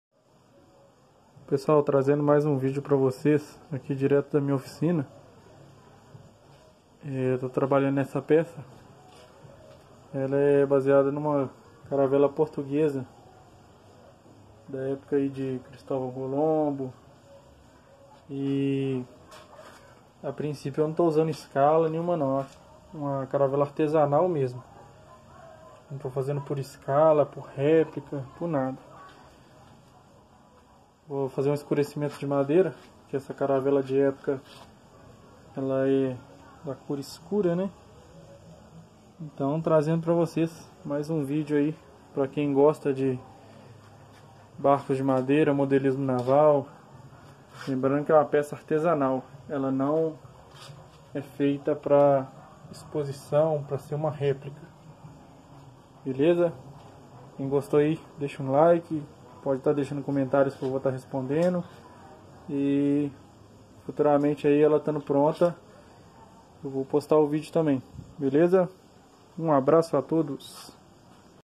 caravela portuguesa em construção
caravela artesanal feita a partir de palitos se picolé.